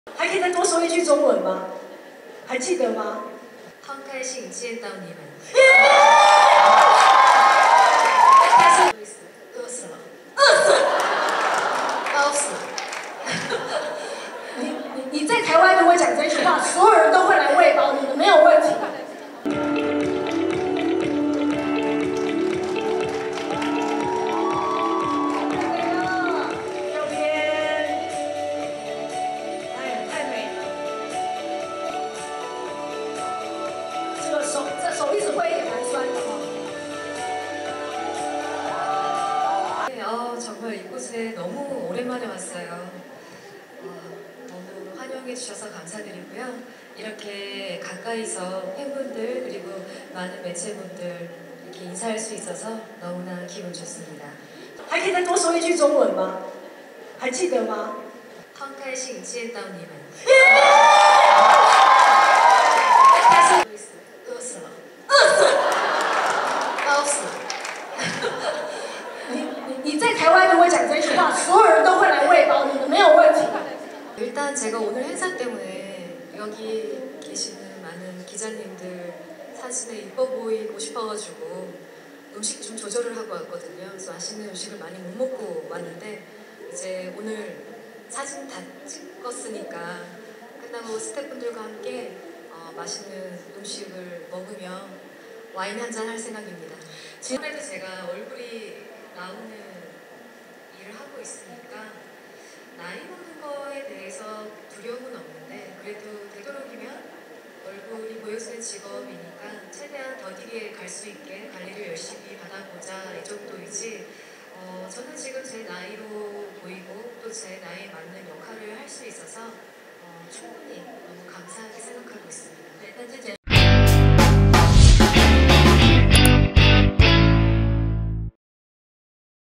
宋慧喬飆中文：餓死了！來台灣不敢吃東西 原因讓人好心疼｜NOWnews
韓星宋慧喬今（24）日來台出席代言記者會，她提到一路走過來，成為了當紅大明星，其實還是會不安與擔憂，特別是選作品時，「我對未來還是有擔憂、焦慮，但抱持期待還是多一點，我對我的未來還是非常期待的。」她也提到今天為了被拍美美，來台灣很節制不敢吃東西，飆中文「很開心見到你們」、「餓死了」。